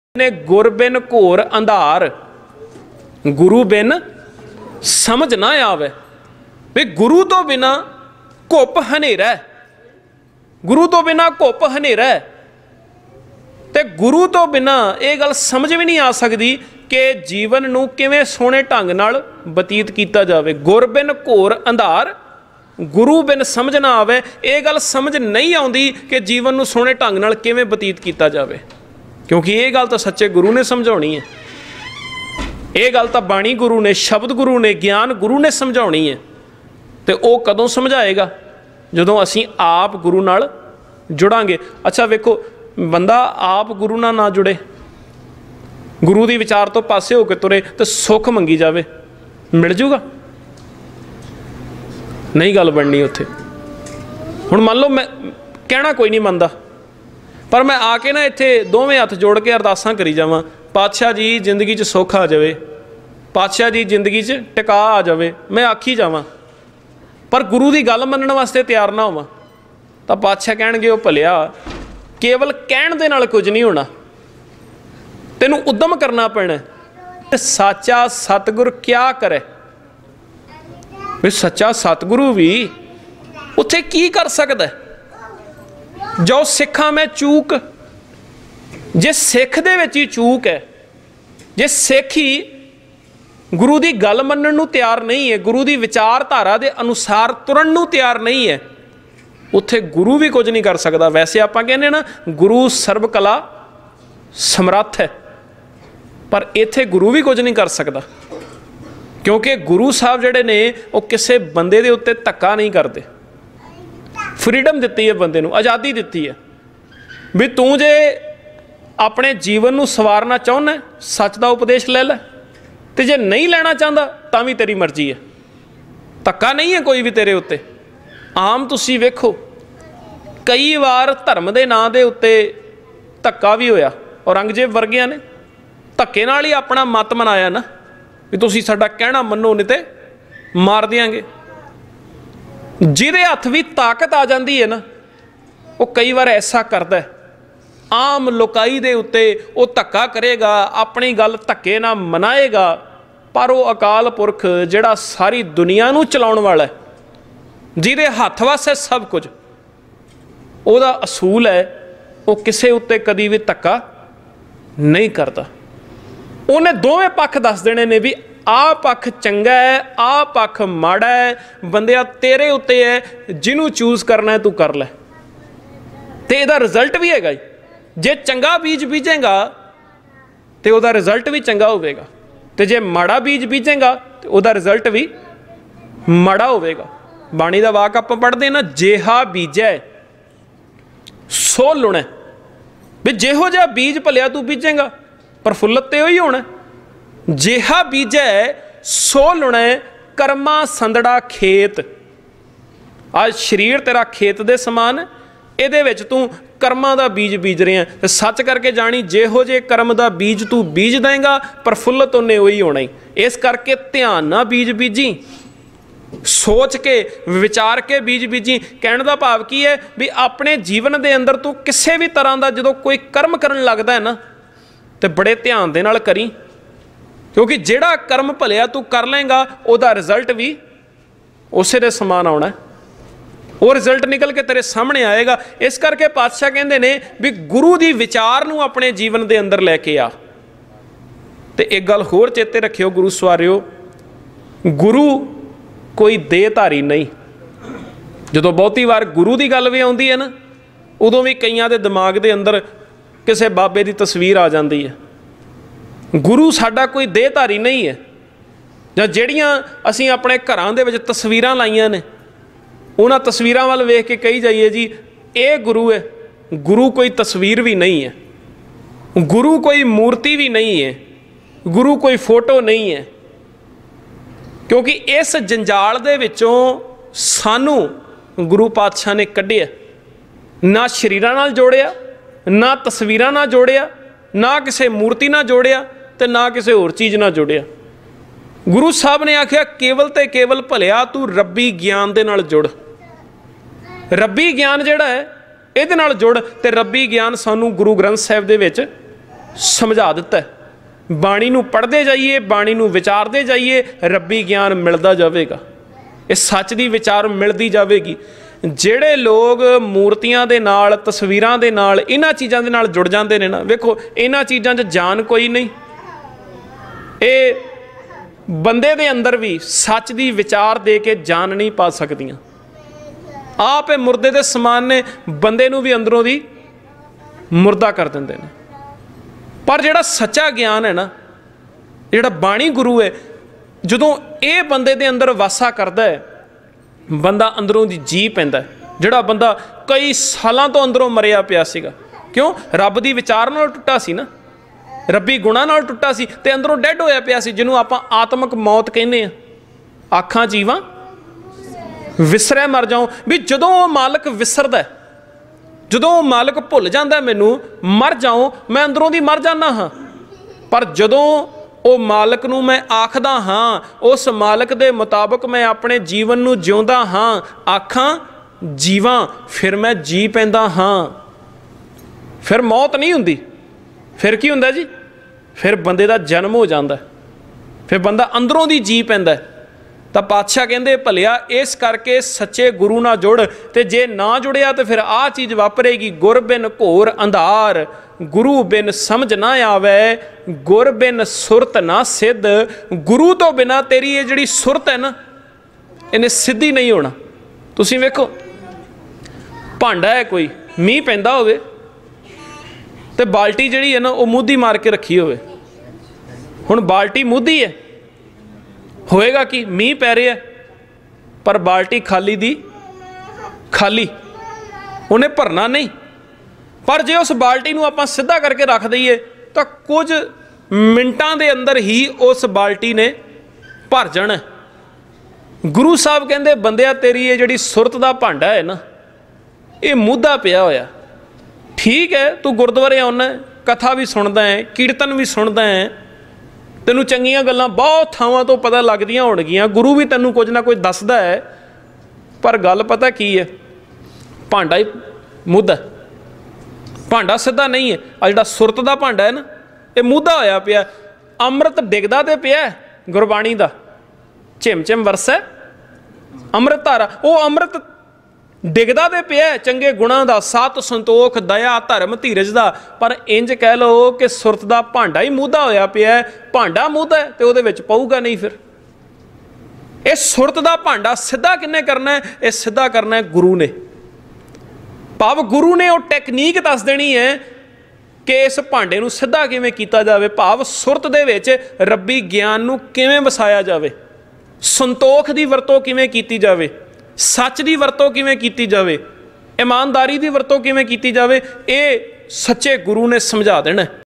"ਅਕਾਲਪੁਰਖ" ਕਦੀ ਵੀ "ਧੱਕੇ" ਨਾਲ ਆਪਣੀ ਗੱਲ ਨਹੀਂ ਮਨਾਉਂਦਾ By Baljeet Singh Delhi (Stuttgart,Germany)
"Akaalpurakh" Kadi Vi "Dhakkey" Naal Aapnni Gal Nahin Manaonda.